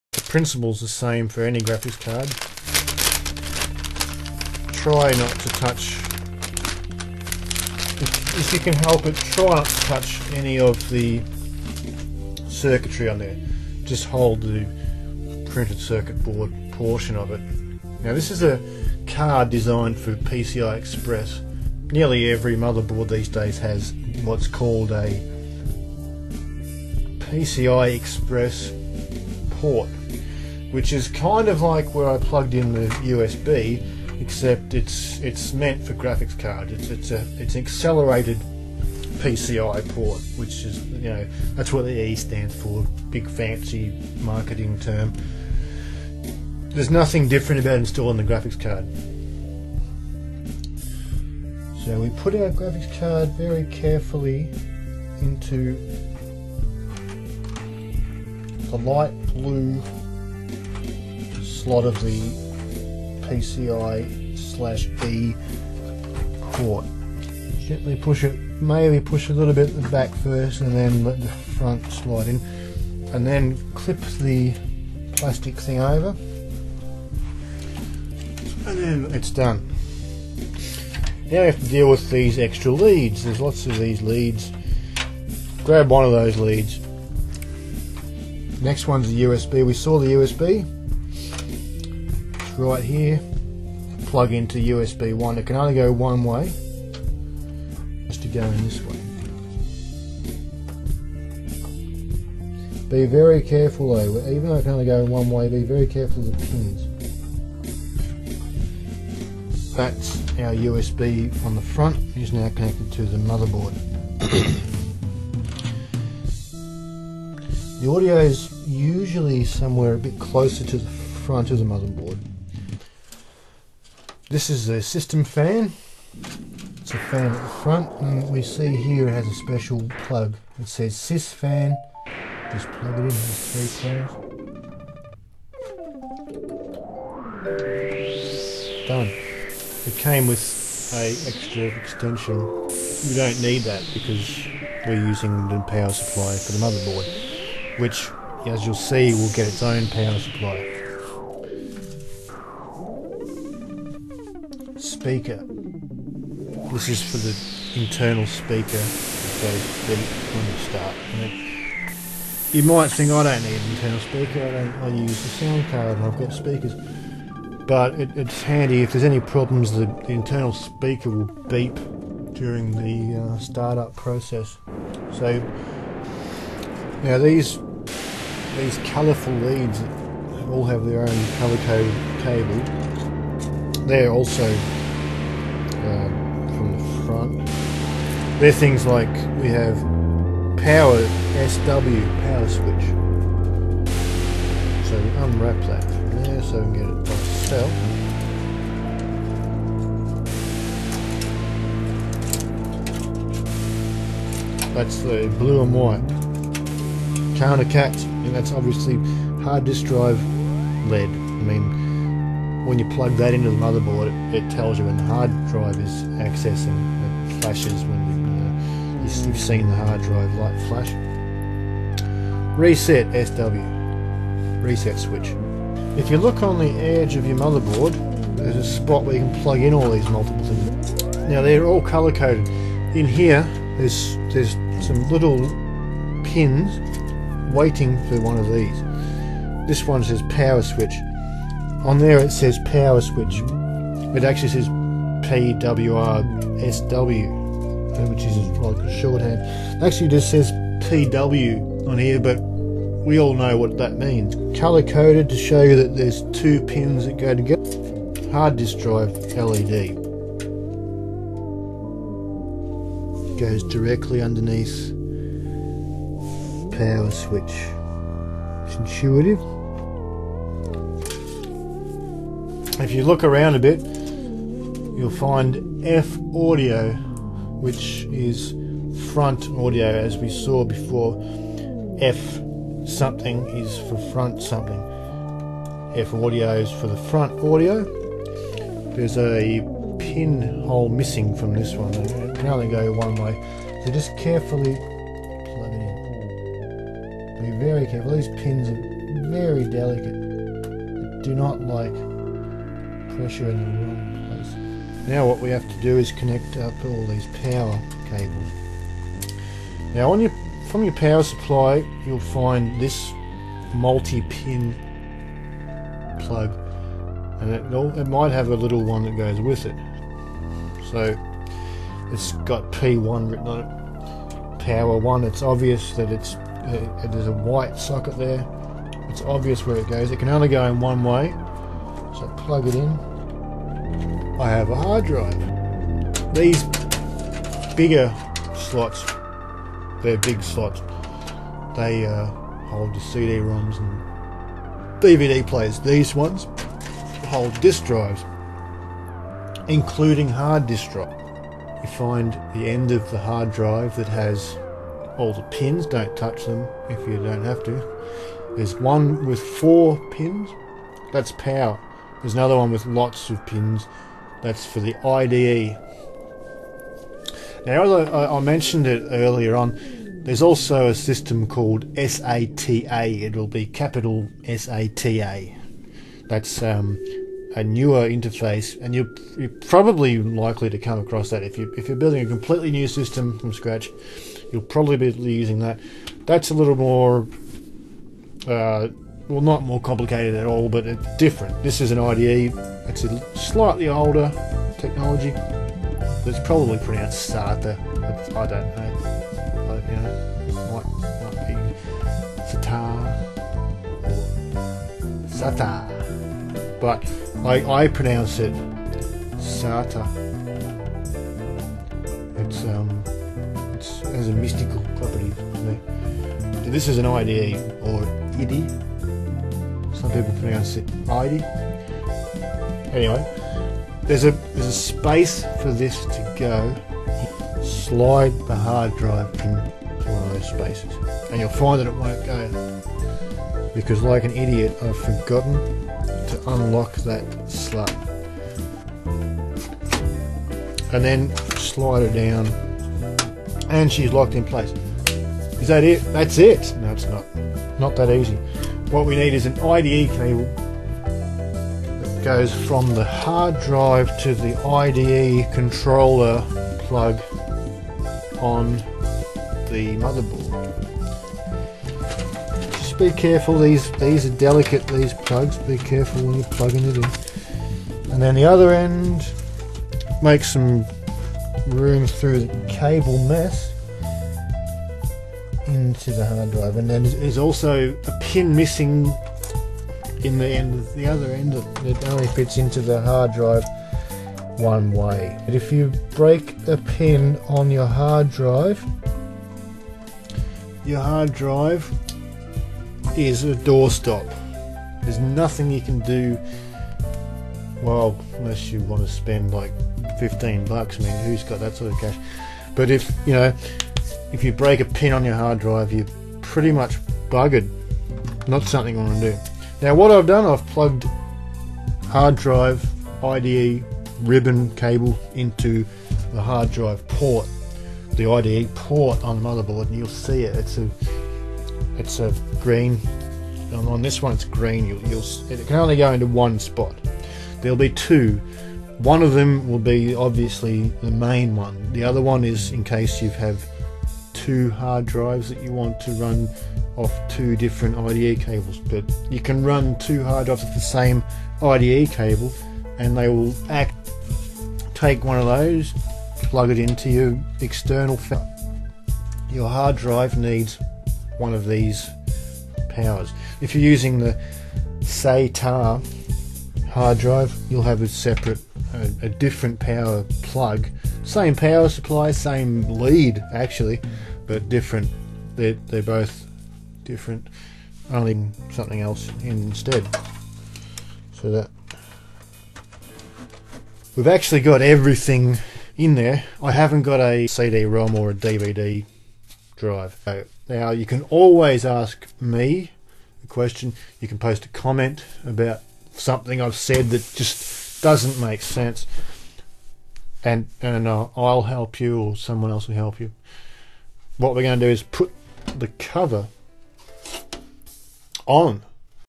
How to Build a PC [PART3 of 3]
If you thought it was too difficult or time consuming, check out this guide. I've aimed this at people who have never done this before.